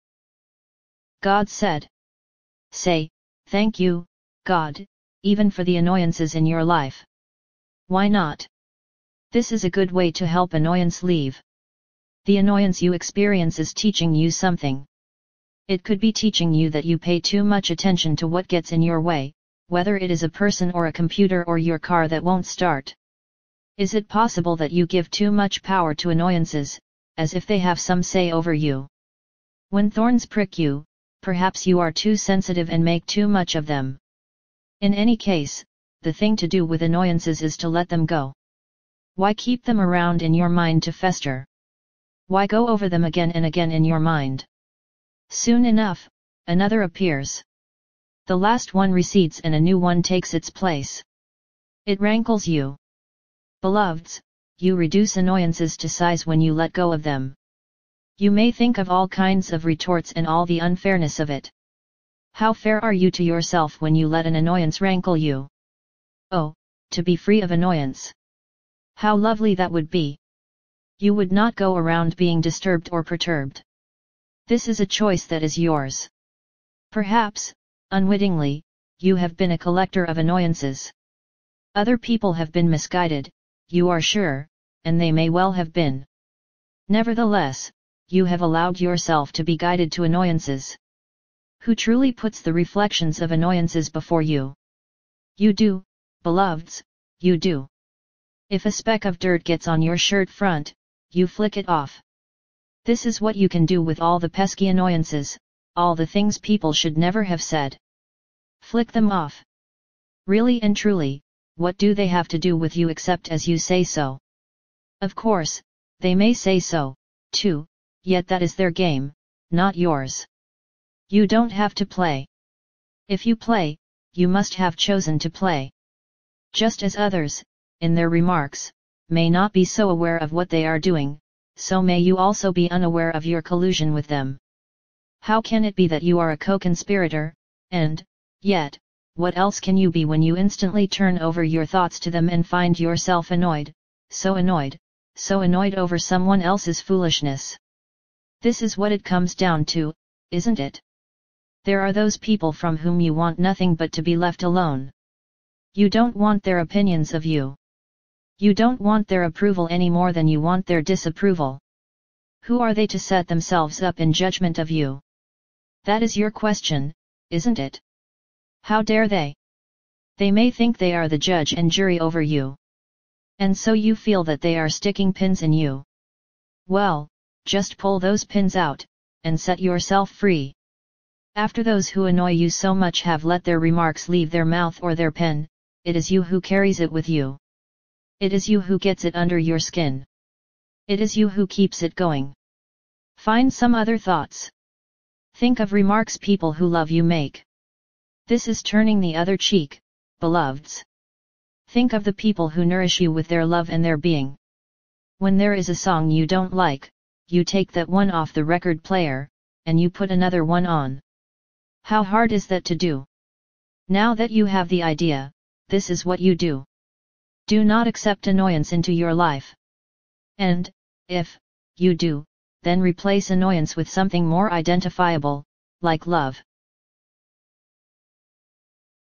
Heaven Letters January-11-2013
Heavenletter #4431 Oh, To Be Free of Annoyance, January 11, 2013
Gloria Wendroff, Godwriter™

God said:
Say, "Thank You, God," even for the annoyances in your life. Why not? This is a good way to help annoyance leave.

The annoyance you experience is teaching you something. It could be teaching you that you pay too much attention to what gets in your way, whether it is a person or a computer or your car that won't start. Is it possible that you give too much power to annoyances, as if they have some say over you?

When thorns prick you, perhaps you are too sensitive and make too much of them. In any case, the thing to do with annoyances is to let them go. Why keep them around in your mind to fester? Why go over them again and again in your mind? Soon enough, another appears. The last one recedes and a new one takes its place. It rankles you.

Beloveds, you reduce annoyances to size when you let go of them.

You may think of all kinds of retorts and all the unfairness of it. How fair are you to yourself when you let an annoyance rankle you?

Oh, to be free of annoyance. How lovely that would be! You would not go around being disturbed or perturbed. This is a choice that is yours. Perhaps, unwittingly, you have been a collector of annoyances. Other people have been misguided, you are sure, and they may well have been. Nevertheless, you have allowed yourself to be guided to annoyances. Who truly puts the reflections of annoyances before you? You do, beloveds, you do.

If a speck of dirt gets on your shirt-front, you flick it off. This is what you can do with all the pesky annoyances, all the things people should never have said. Flick them off. Really and truly, what do they have to do with you except as you say so? Of course, they may say so, too, yet that is their game, not yours. You don't have to play. If you play, you must have chosen to play. Just as others, in their remarks, may not be so aware of what they are doing, so may you also be unaware of your collusion with them. How can it be that you are a co-conspirator, and, yet, what else can you be when you instantly turn over your thoughts to them and find yourself annoyed, so annoyed, so annoyed over someone else's foolishness. This is what it comes down to, isn't it?

There are those people from whom you want nothing but to be left alone. You don't want their opinions of you. You don't want their approval anymore than you want their disapproval. Who are they to set themselves up in judgment of you? That is your question, isn't it? How dare they? They may think they are the judge and jury over you. And so you feel that they are sticking pins in you. Well, just pull those pins out, and set yourself free. After those who annoy you so much have let their remarks leave their mouth or their pen, it is you who carries it with you. It is you who gets it under your skin. It is you who keeps it going.

Find some other thoughts. Think of remarks people who love you make. This is turning the other cheek, beloveds. Think of the people who nourish you with their love and their Being.

When there is a song you don't like, you take that one off the record player, and you put another one on. How hard is that to do?

Now that you have the idea, this is what you do. Do not accept annoyance into your life. And, if, you do, then replace annoyance with something more identifiable -- like love.

Heavenletters™ -- Helping Human Beings Come Closer to God and Their Own Hearts
Gloria Wendroff, Godwriter™